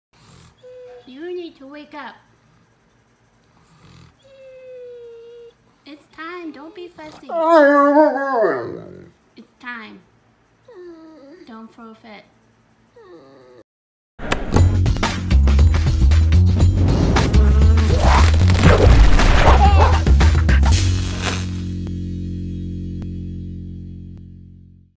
Sleepy Newfoundland refuses to get out of bed
This giant 160 lbs Newfoundland has taken over the bed and won't get up to start the day. He needs his beauty sleep! He is so adorable!